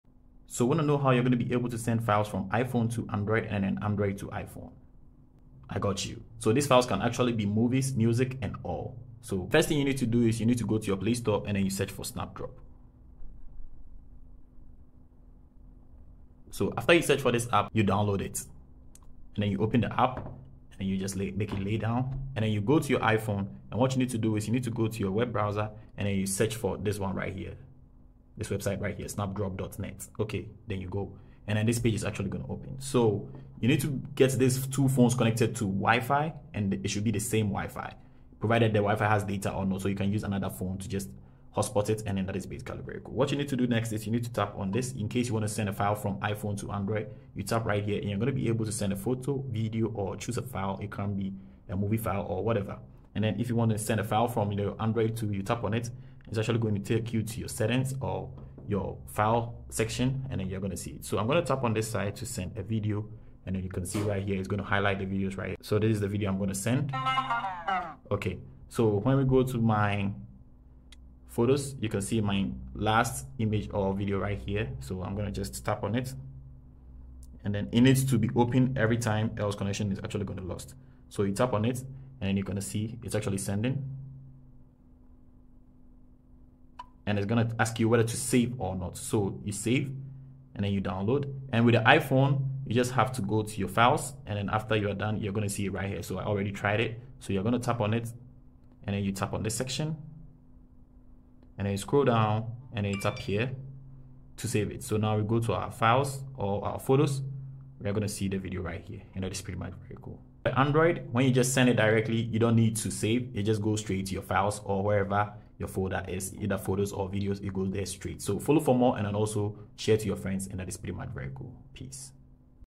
How To Send Files From iPhone To Android Phone. (Easiest Method)
In this video you will be able to know how you can send files from Android to iphone and vice versa.
Even you can use this basic steps to send files from both iPhone and Android to a computer too.

Phone Cases I Use
Rugged Shield Cases :